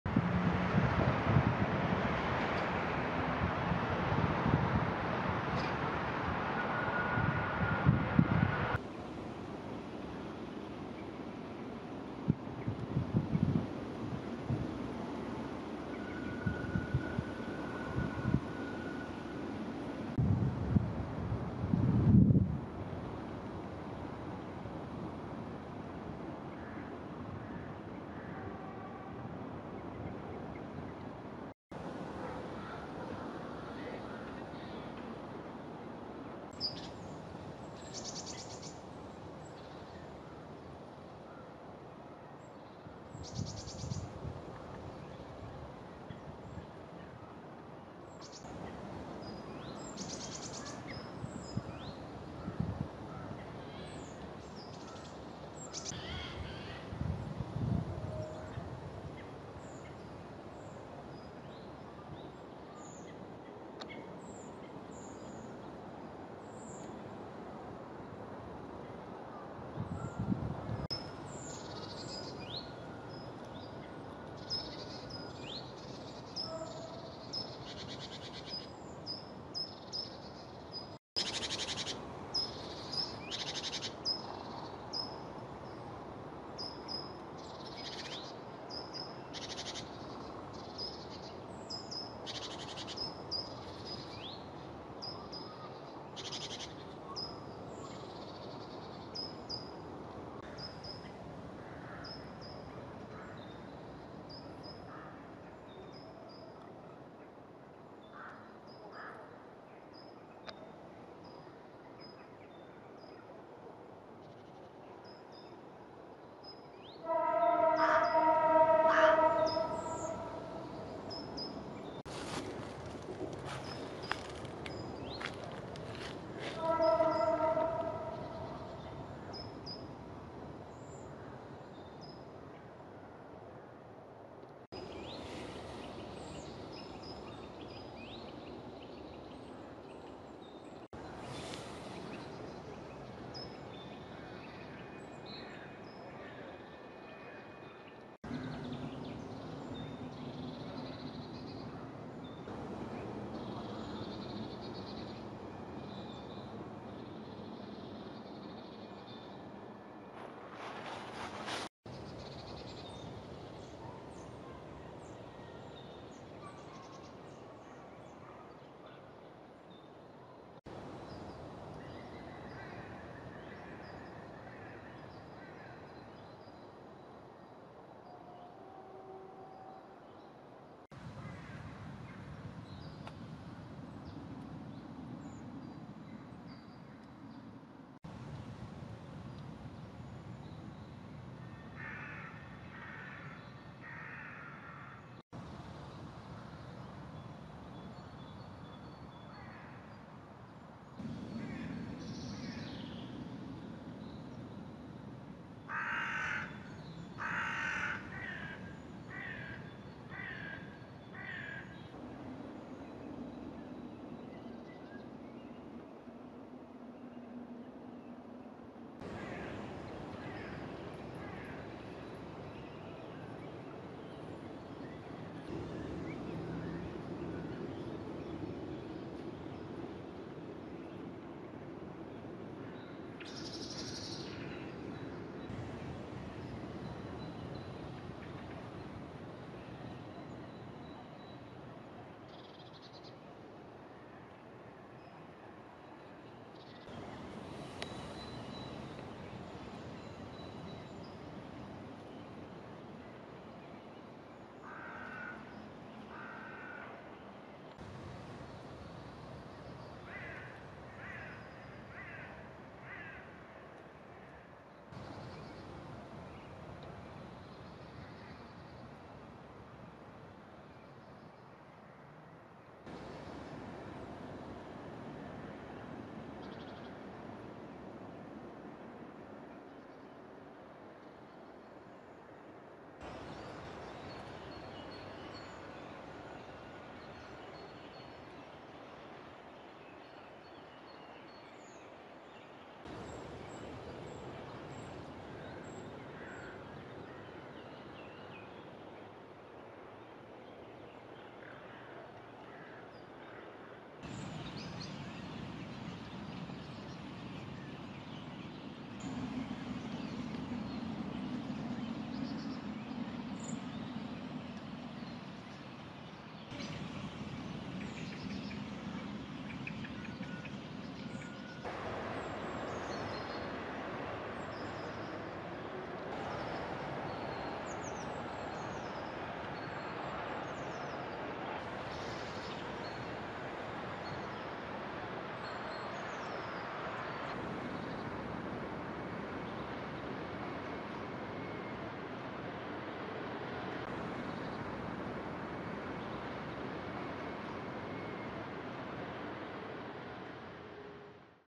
Lasek Oporowski i okolice - Wrocław - luty 2023.
Impresja przyrodnicza zrealizowana 15 lutego 2023 we Wrocławiu, w Lasku Oporowskim i okolicy.